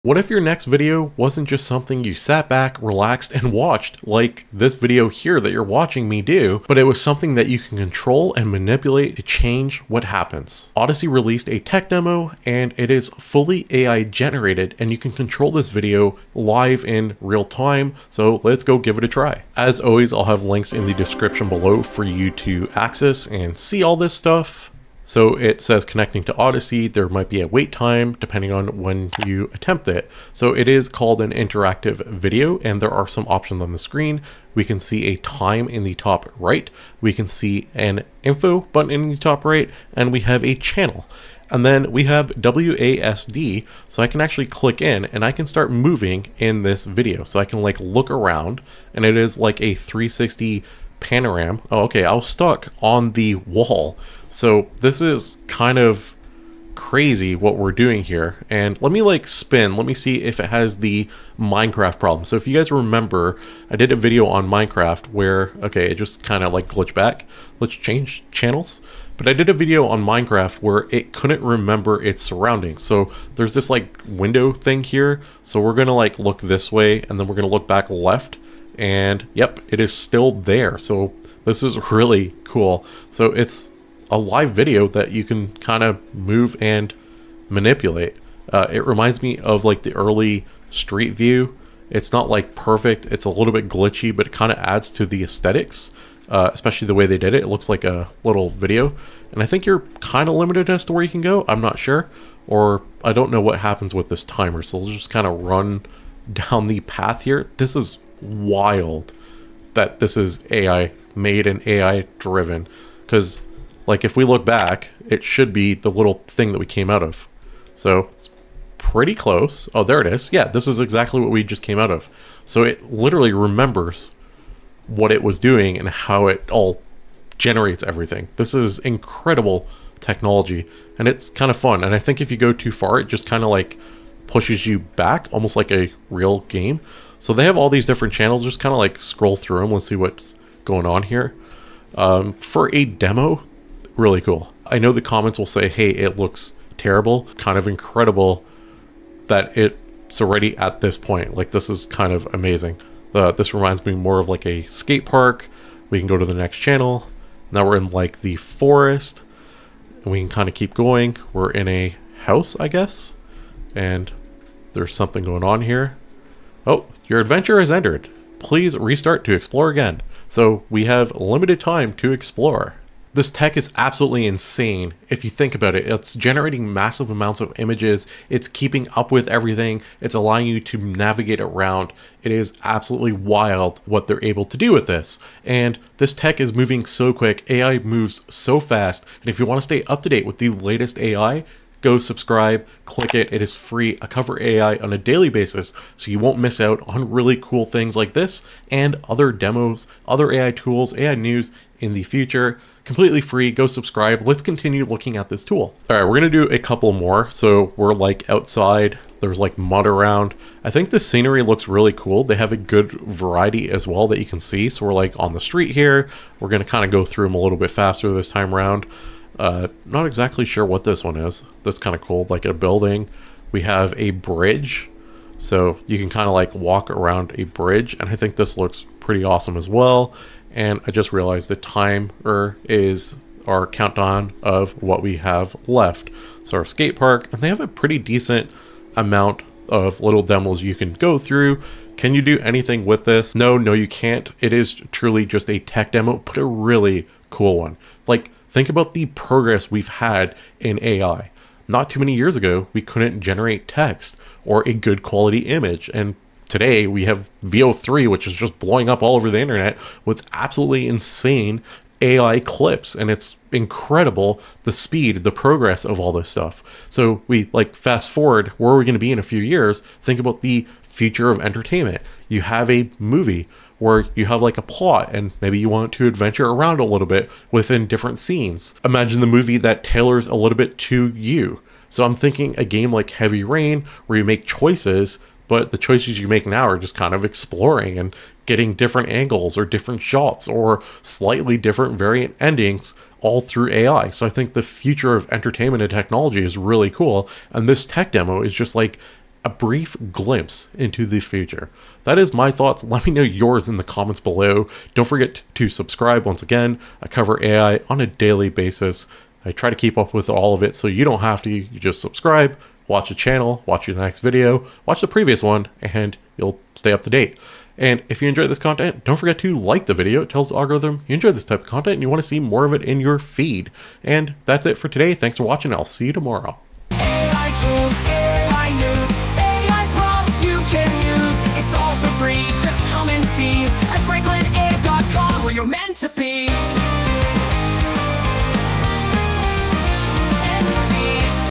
AI Powered Interactive Videos! 🤯
Control Interactive AI Videos in Real-Time!

Imagine watching a video you don’t just passively view but actively control in real time! In this demo of Odyssey's groundbreaking AI technology, explore a fully interactive, AI-generated video experience that puts you in charge. Navigate immersive 360-degree environments, switch between unique channels, and marvel at AI's ability to remember and adapt to your movements. It’s like stepping into a futuristic game or movie where you dictate the story.

From glitchy-yet-charming aesthetics reminiscent of early Street View to mind-blowing innovations like dynamic scene generation, this tech demo offers a sneak peek into the next level of entertainment and creativity. This is more than AI—it’s the future of technology shaping how we experience digital content.

CHAPTERS:
00:00 - Introducing Odyssey: The Future of 3D!
00:17 - First Look: Interactive World Demo
00:43 - Night City: Urban Exploration
01:07 - Into the Mist: Natural Landscapes
01:56 - AI Architecture: Diverse Designs
02:21 - World Hopping: Rapid Scene Generation
02:52 - Odyssey's Vision: Revolutionizing 3D
03:25 - Under the Hood: Real-Time Streaming Tech
04:02 - Endless Possibilities: What's Next for Odyssey?
05:26 - Important Stuff